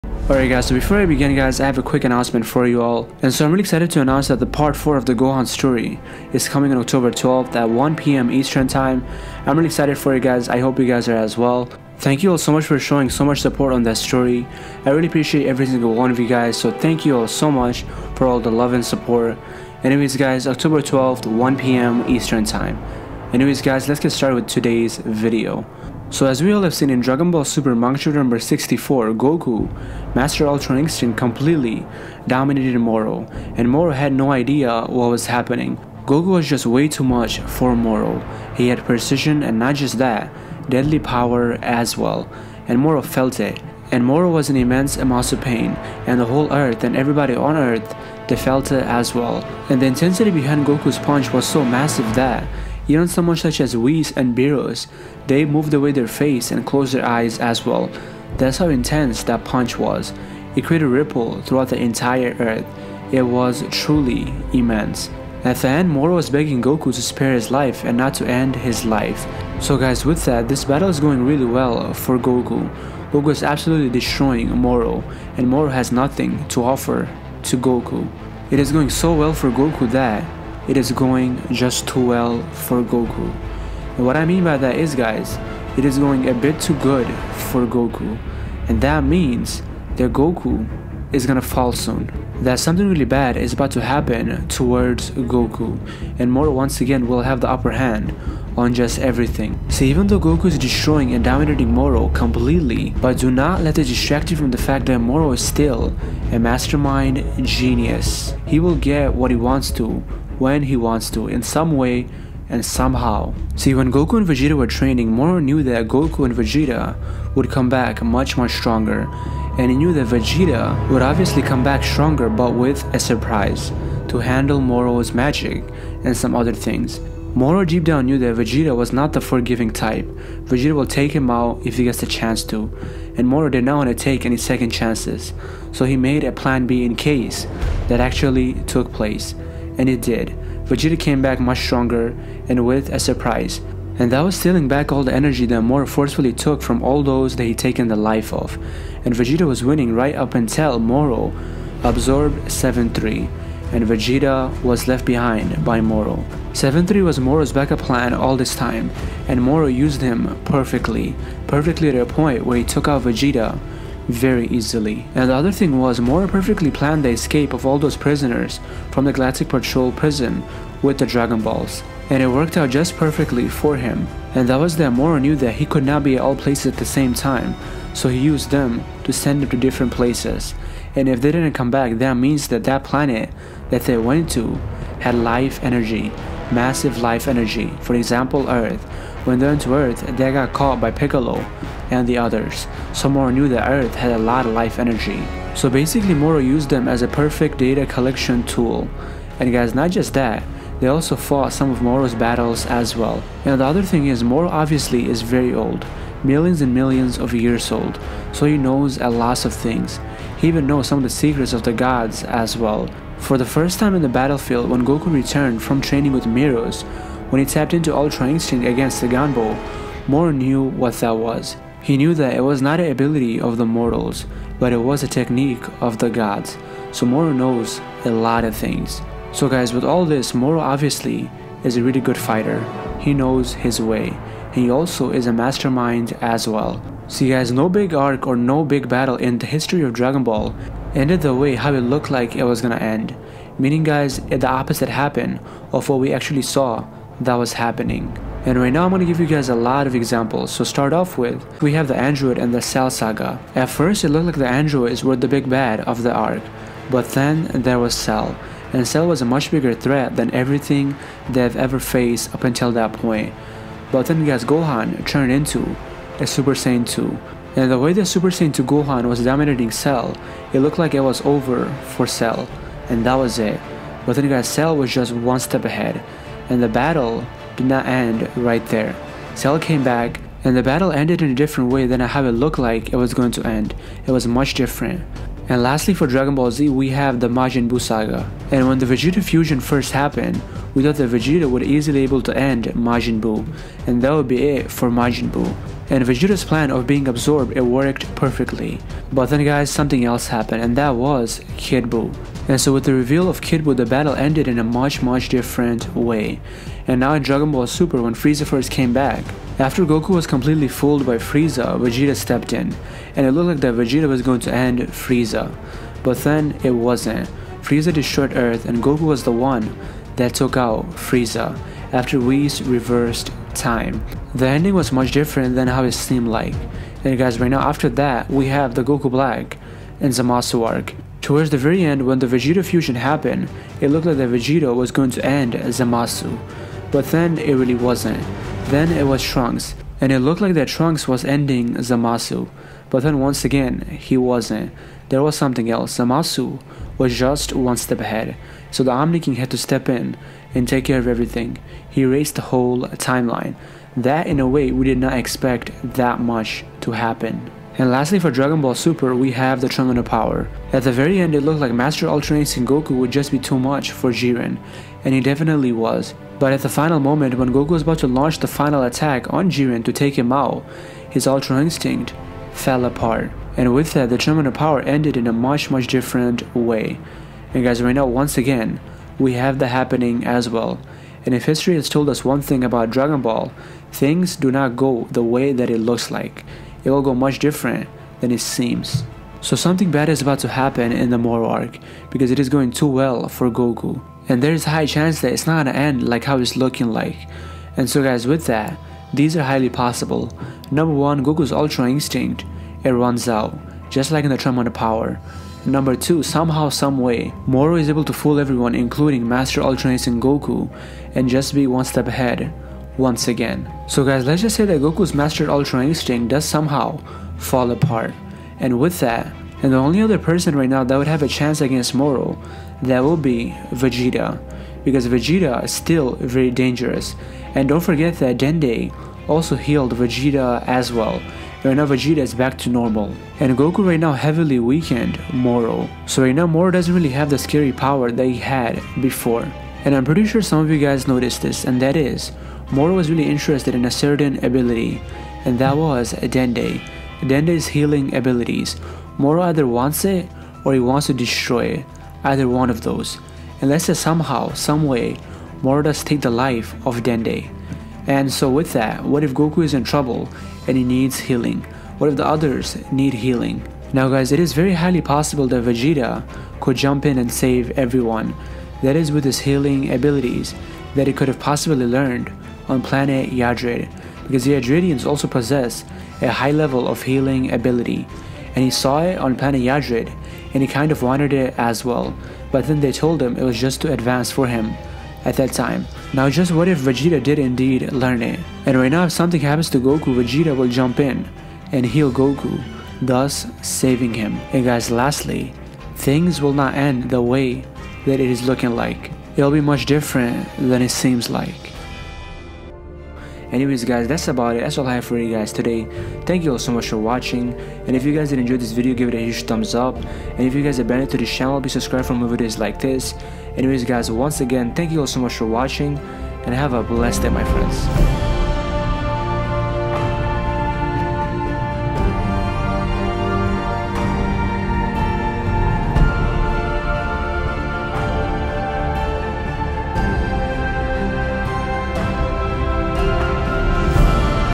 Goku’s FINAL STAND with Moro Fails, Vegeta Saves Goku From Moro!? (Unexpected Twist is COMING)
Vegeta SAVES Goku's Life After Moro does Something Unexpected? (Vegeta's 2nd Hidden Ability)

Gohan Part 4! is coming on Oct 12th at 1:00 PM EST